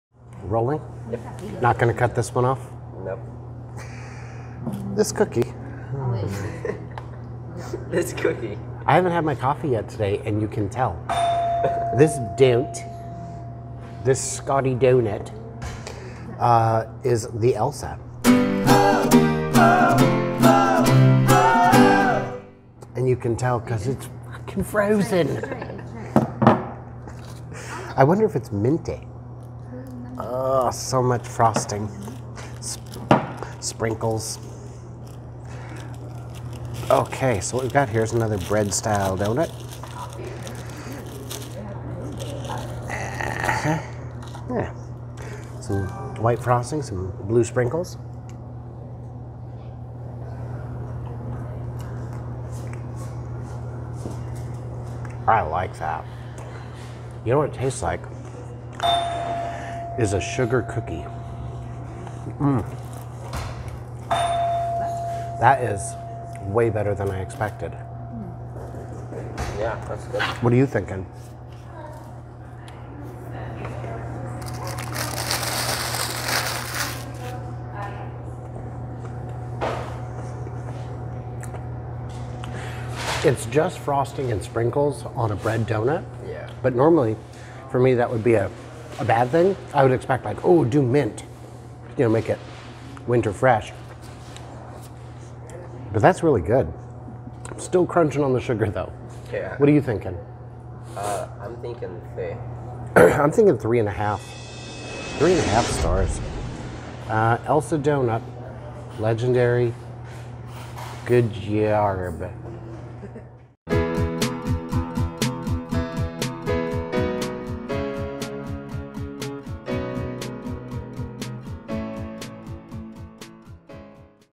What would be an Elsa doughnut if it isn't effing FROZEN!?!
Legendary Doughnuts in South King County (just south of Seattle) is one of the few boutique eateries we truly love. They're a smidge on the premium side, and priced accordingly, but they're so tasty, and the staff is so friendly.

So if you want to check out some exceptional doughnuts in a wide variety of ever-changing flavors and formulas, this is indeed the place to go.

If you want to collaborate or get reviewed, get in touch.